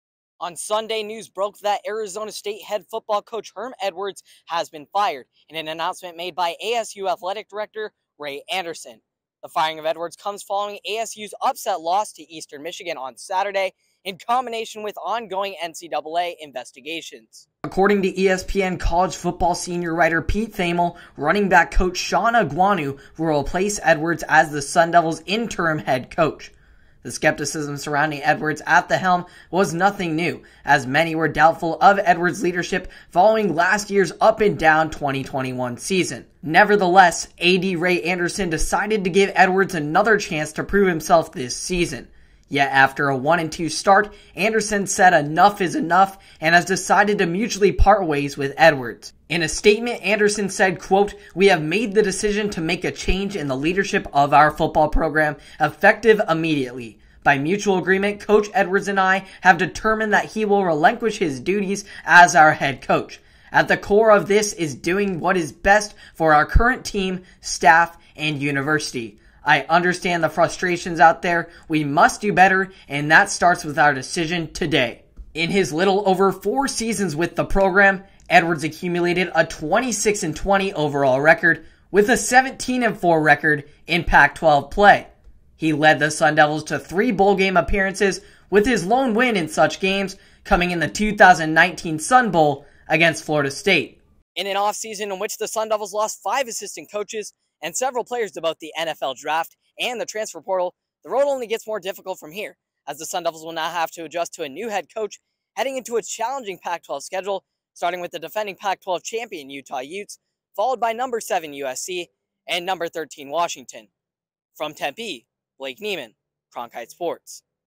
Herm Edwards Out as ASU Head Coach
Breaking News: Arizona State and Head Football Coach Herm Edwards have parted ways.